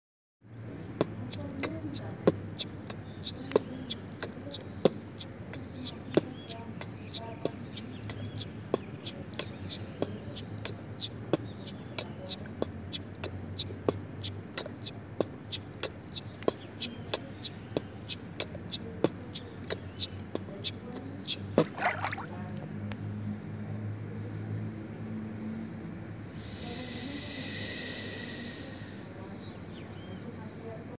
Caleidoscopio Serie del Agua
Grabado en la piscina de un hotel de Posadas Misiones